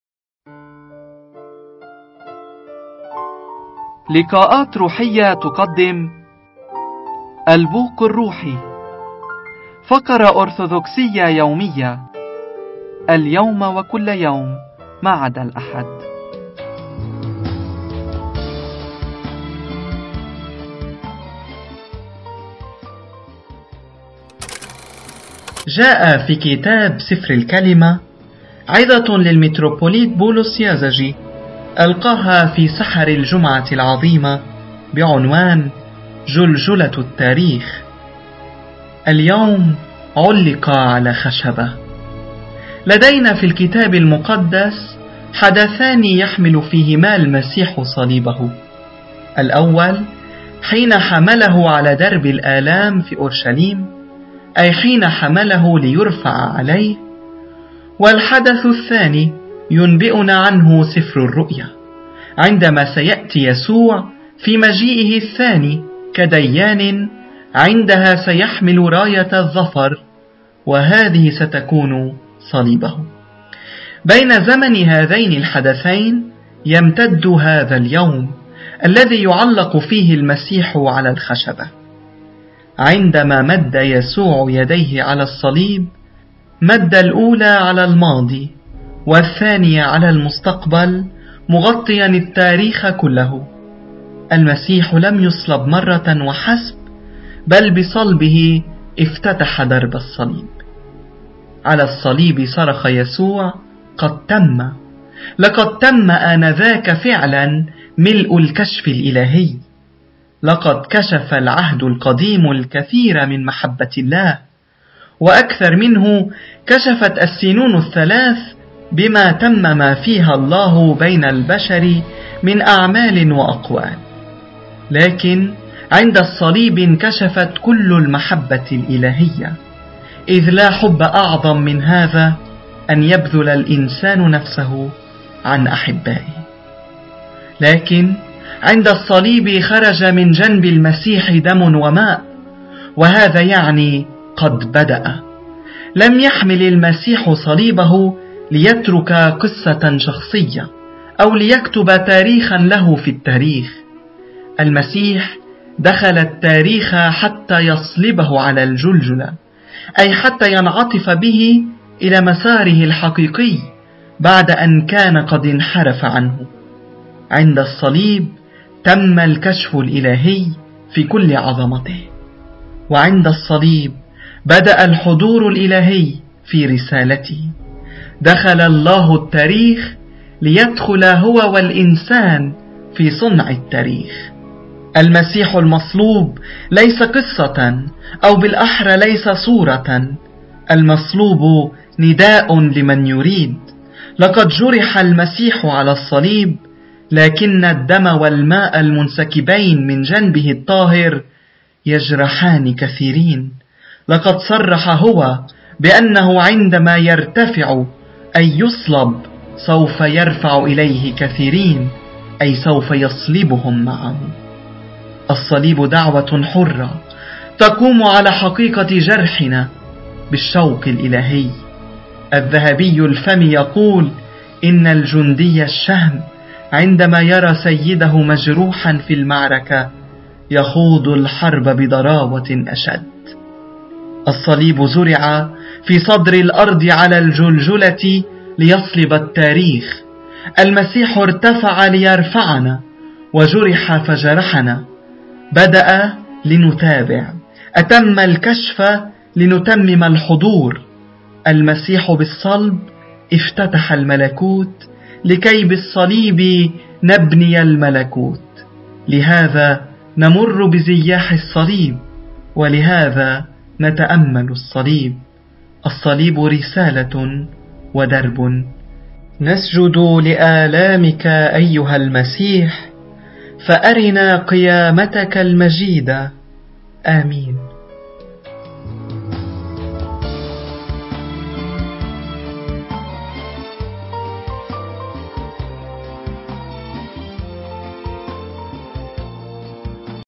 الجمعة العظيمة جلجلة التاريخ.
☦ لـِقَـاءَات رُوحـِيـَّـة ☦
             تُقَدِّمُ:
ا🔔🔹 ⛪ 🔹🔔ا
🙏       خِدْمـَةَ      🙏
⛪ البـوق الرّوحـي ⛪
🙏      اليَوْميَّة      🙏
ا🔔🔹 ⛪ 🔹🔔ا

في رَقَمِهِ التَّسَلْسُليّ:  2554

يَوْمَ الجُمْعَةِ الوَاقِعِ فِيهِ:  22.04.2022

بِصَوْتِ : قدس الأب سيرافيم مخول.

بِعِنْوان: الجمعة العظيمة جلجلة التاريخ.